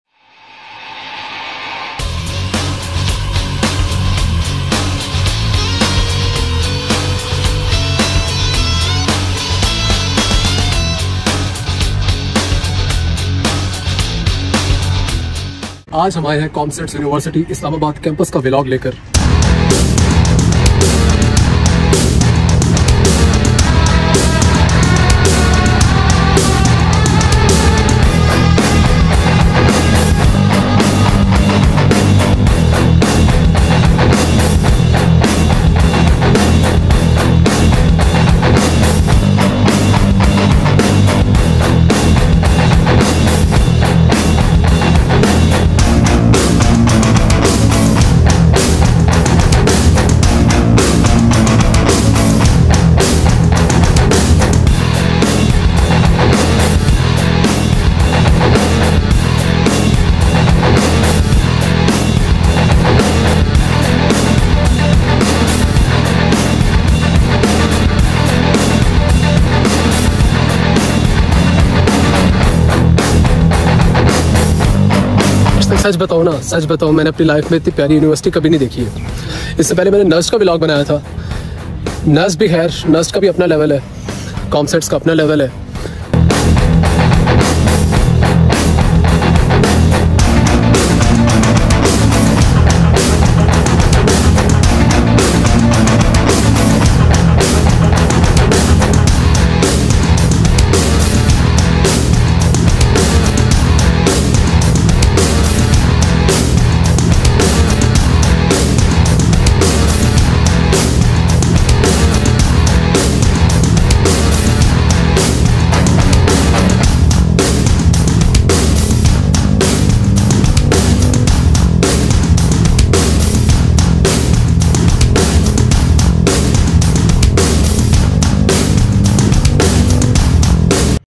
Comsats University Islamabad | Complete Video Link in Description 👇👇
Complete Tour & Information of about COMSATS University Islamabad Campus